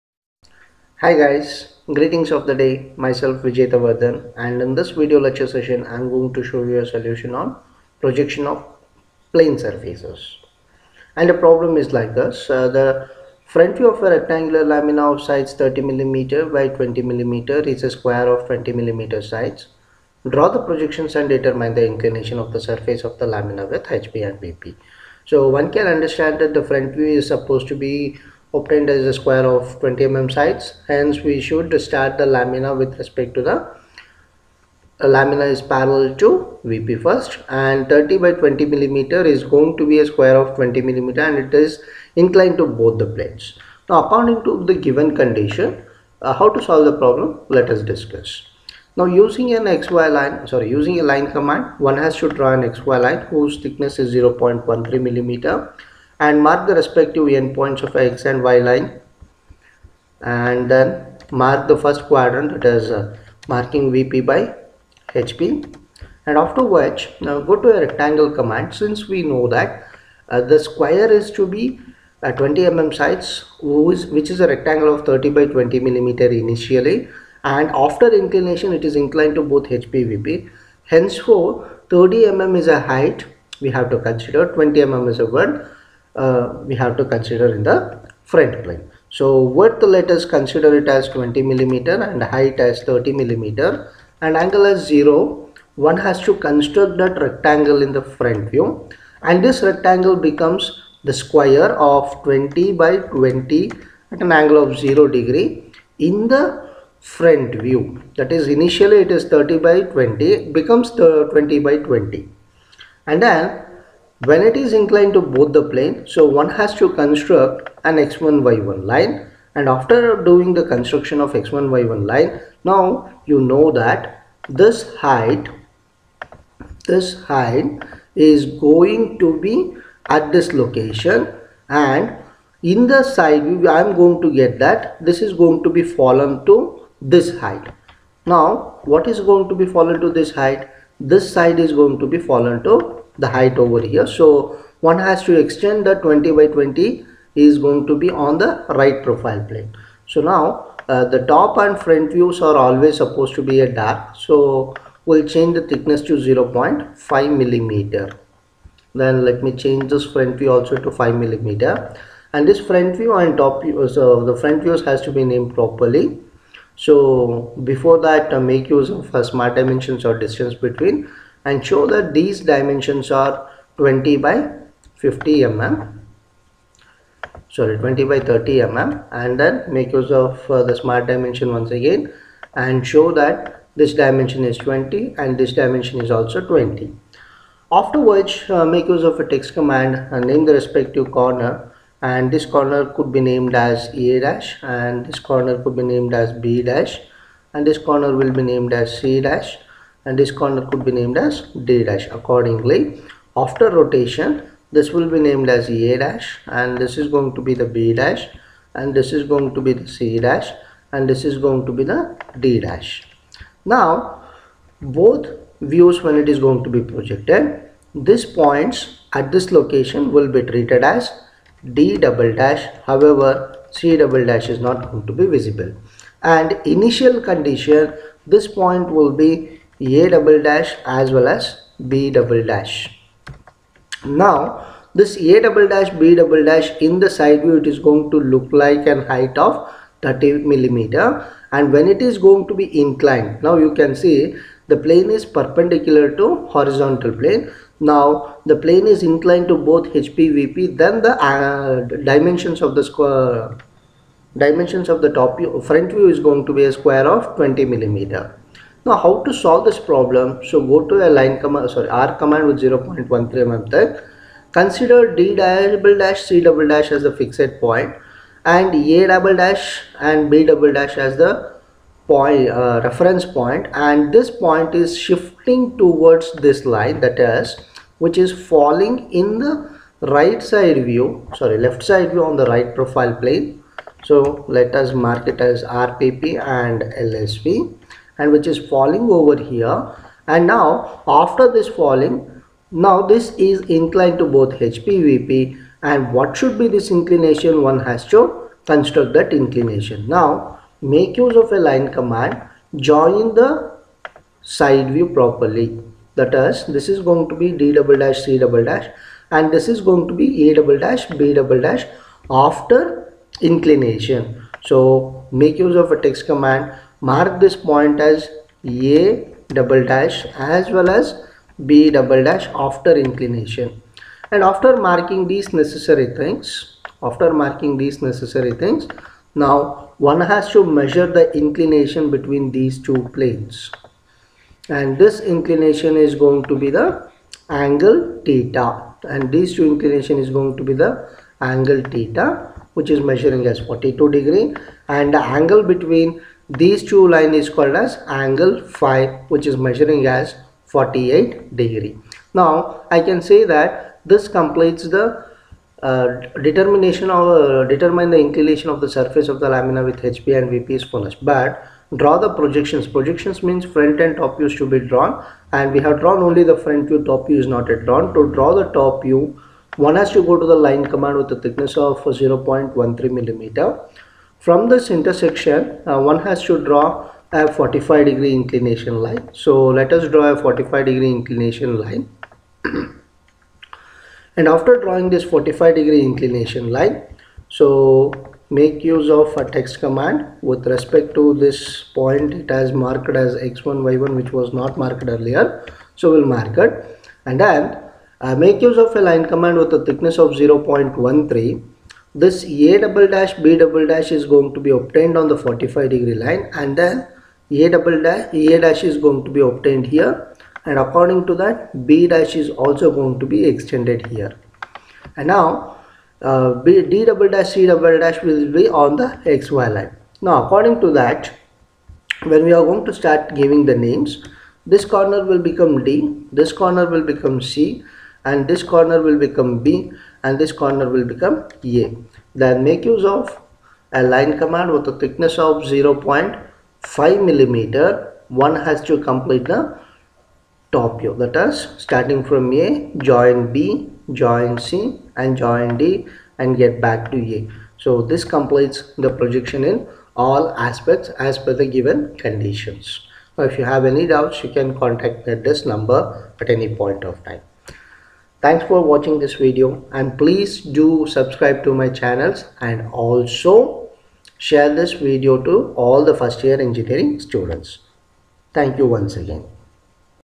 Example 16 on Projection of Plane Surfaces
The front view of a rectangular lamina of sides 30 mm X 20 mm is square of 20 mm sides. Draw the projections and determine the inclinations of the surface of the lamina with HP and VP.